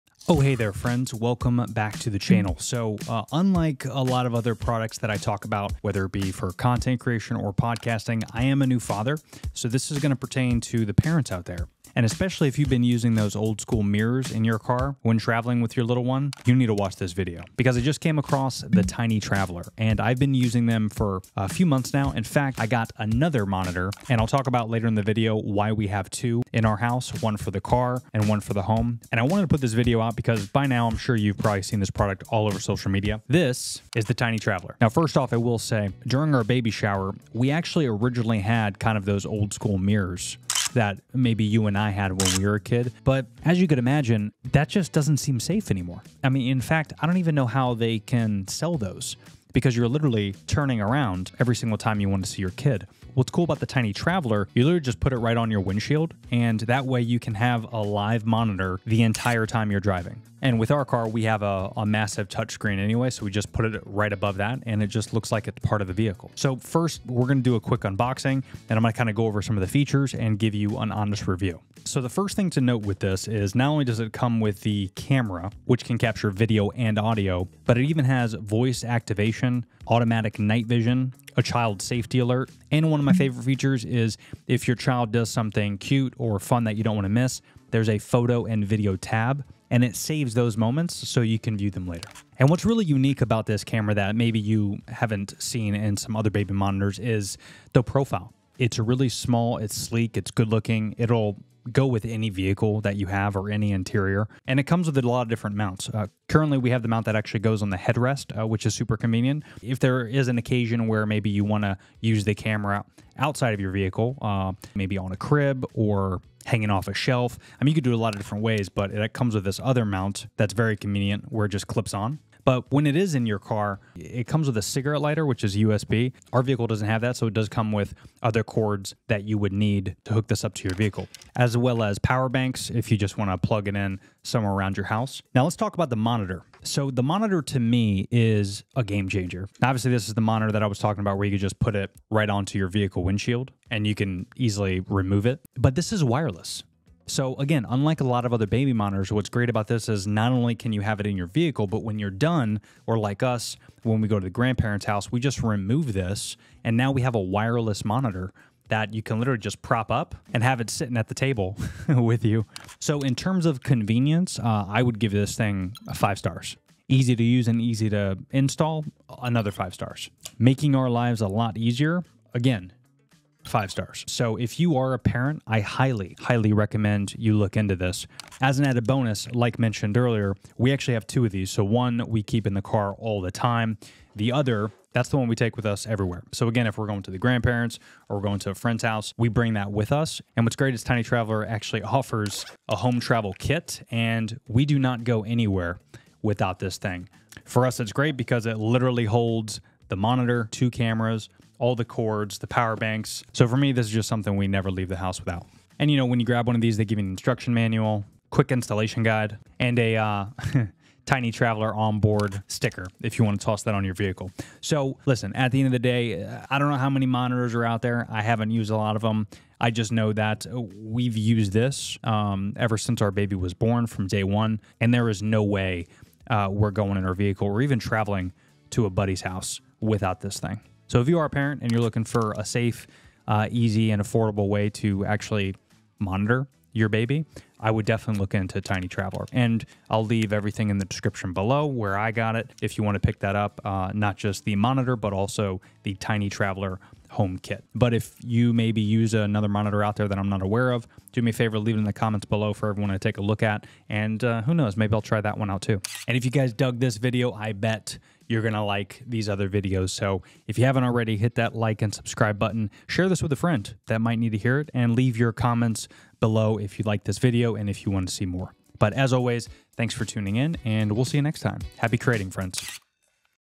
DON'T BUY THIS BABY MONITOR (before you watch this)!
Pros and Cons of this babymonitor but is it worth the investment? All that and more in this quick review of the TINY TRAVELER!

🍼Tiny Traveler Baby Monitor:

🍼Tiny Traveler Travel Kit

► FREE ECAMM LIVE TRIAL!
This is what I use for all my videos, podcasts and live streaming.